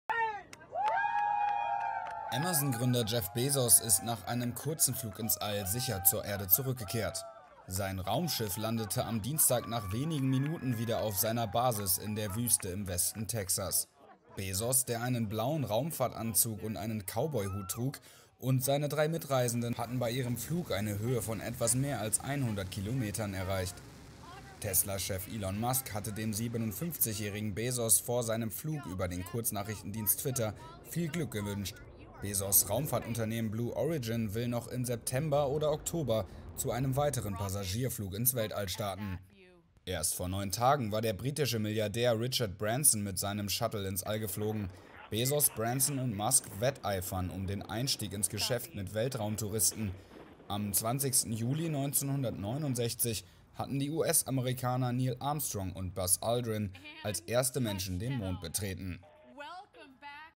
Bezos' Ausflug ins All dauert nur wenige Minuten
Innerhalb nur weniger Minuten hat das Raumschiff des Amazon-Gründers am Dienstag eine Höhe von etwa 100 Kilometer erreicht. Bereits im September oder Oktober soll ein weiterer Passagierflug seines Raumfahrtunternehmen Blue Origin ins Weltall starten.